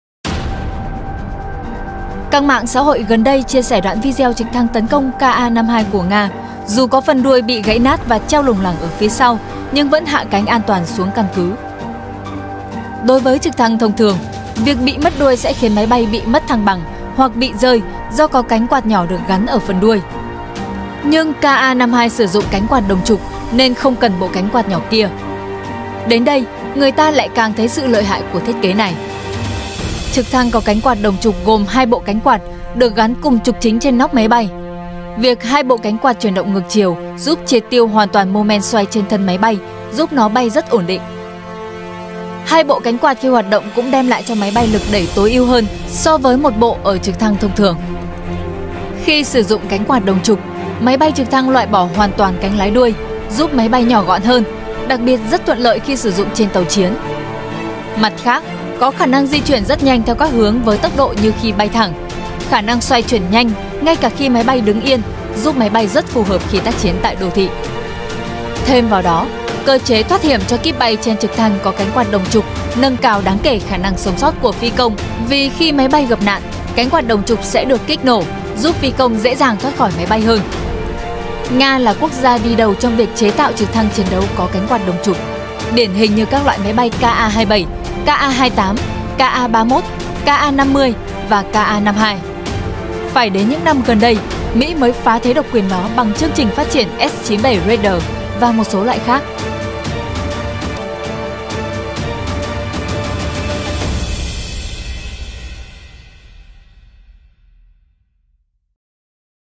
Tại sao trực thăng Ka-52 gãy đuôi vẫn hạ cánh an toàn? - Báo QĐND
Báo QĐND - Các mạng xã hội gần đây chia sẻ đoạn video trực thăng tấn công Ka-52 của Nga dù có phần đuôi bị bị gãy nát và treo lủng lẳng ở phía sau, nhưng vẫn hạ cánh an toàn xuống căn cứ.